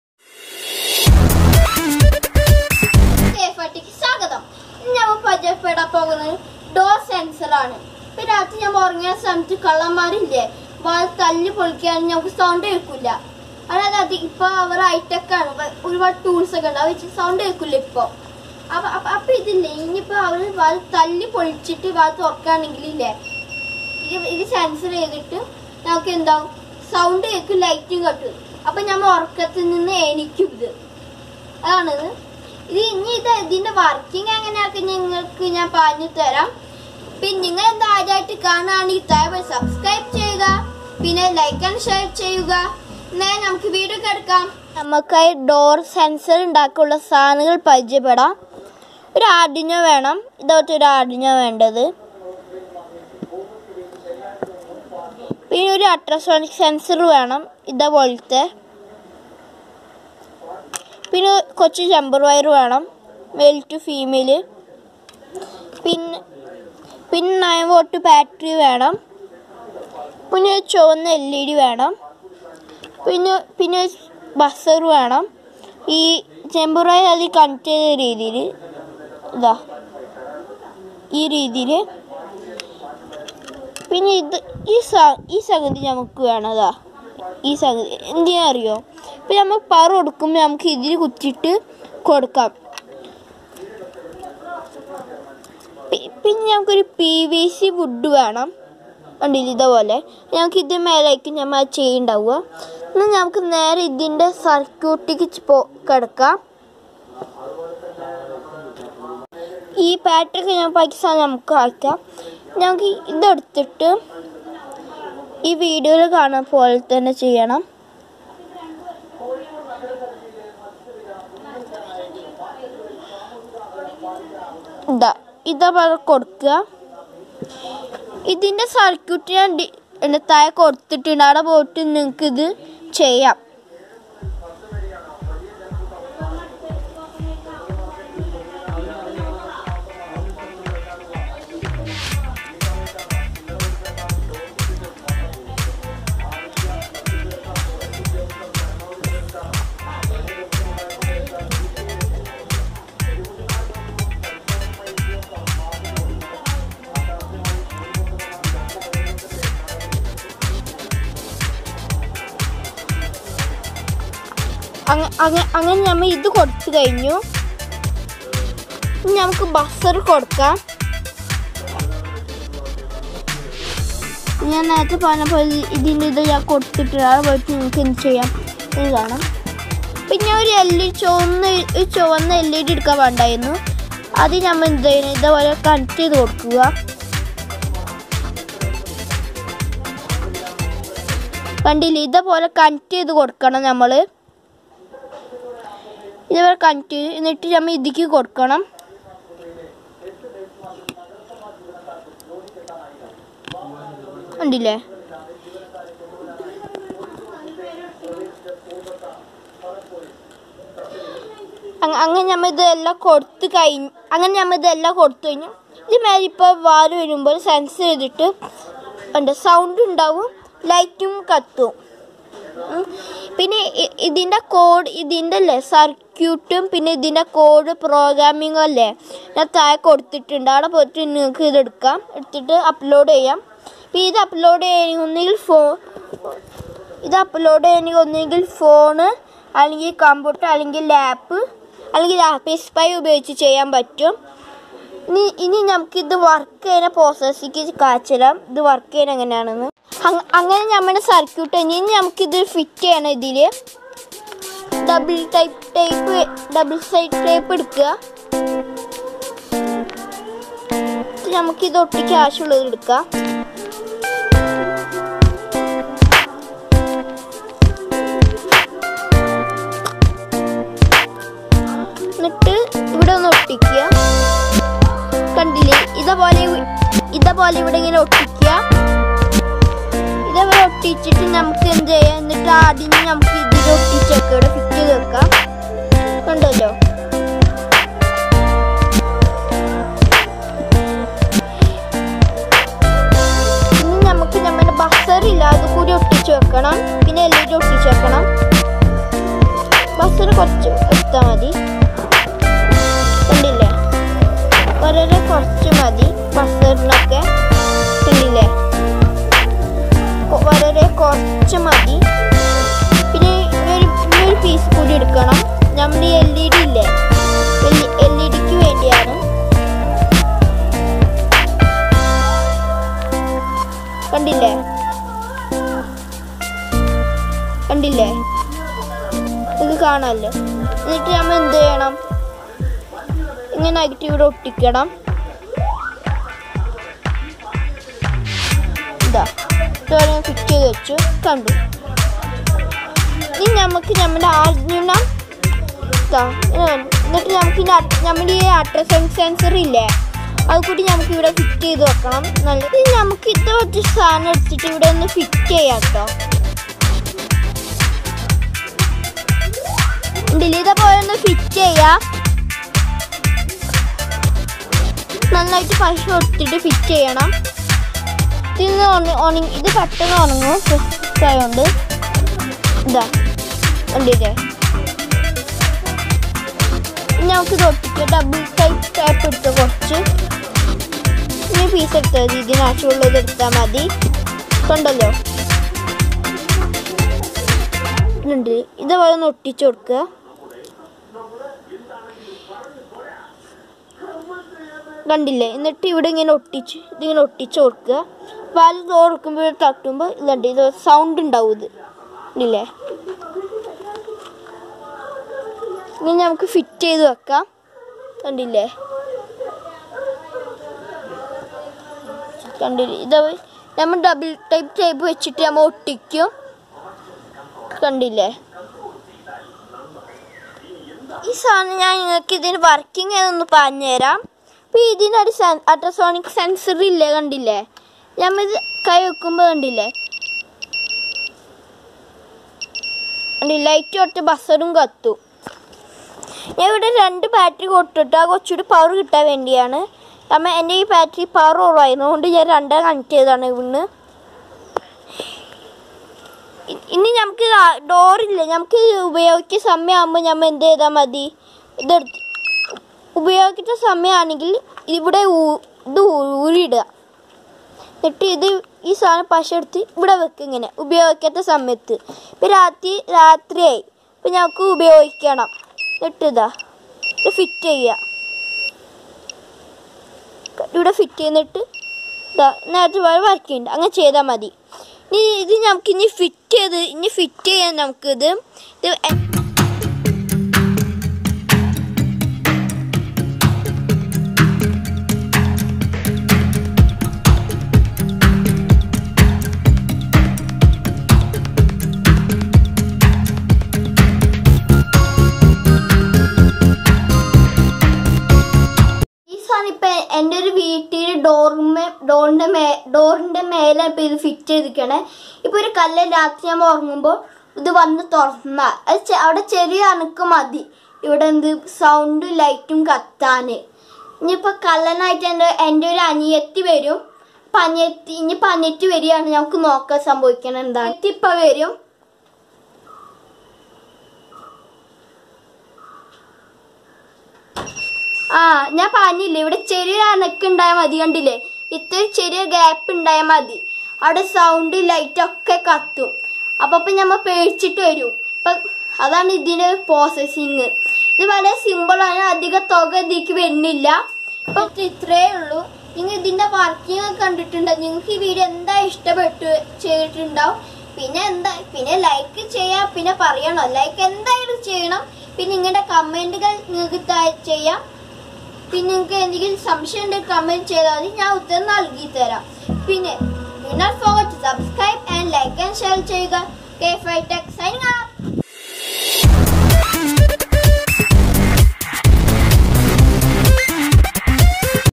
How to make a Door sensor in Malayalam
Hi friends,
   Today I show you how to make a Door sensor at home.

Things to make.

1.Arduino UNO R3 SMD with cable:roboCraze Arduino UNO R3 SMD Development Board with USB cable
2.Ultrasonic :Robo India Ultrasonic Ranging Module HC - SR04 for Arduino, Raspberry Pi, AVR, 8051, PIC and other MCU
3.red LED.
4.DC Bazzer.
5.PVC wood.
6.jumper wire.
7.9V bettery.

K5 tech Naval.